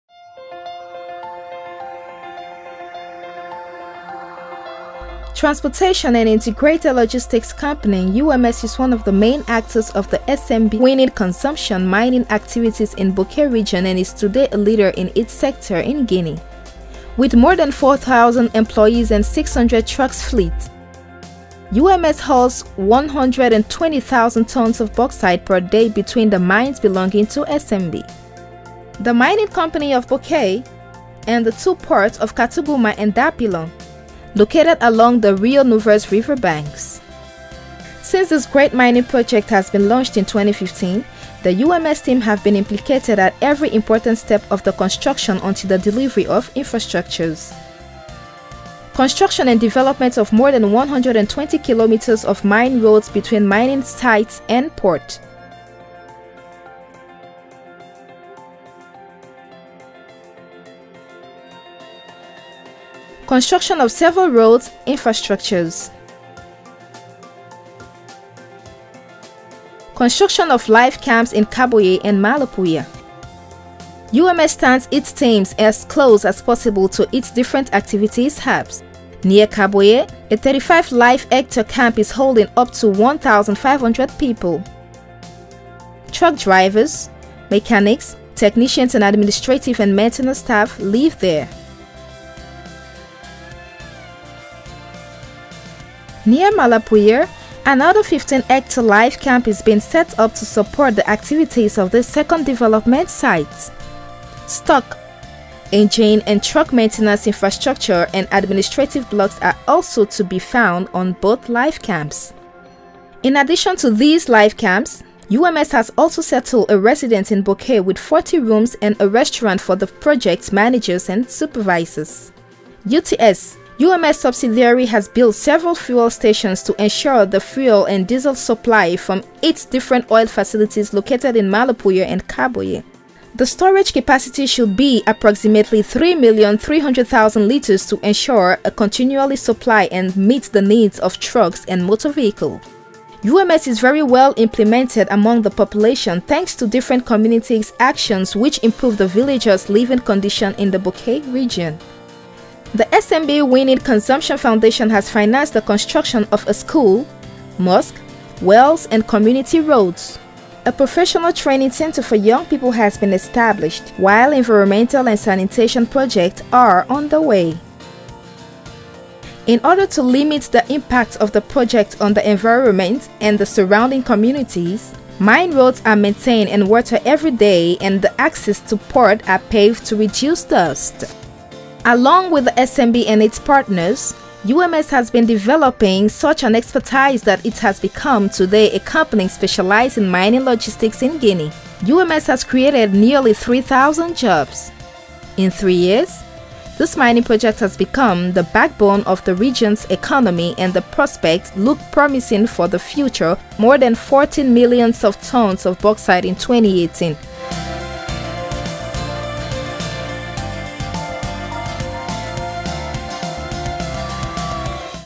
UMS and the SMB WINNING Consortium Project - Feb 2018
UMS and the project have gone even further since our last update in 2017 ! Discover our latest breakthroughs in this video.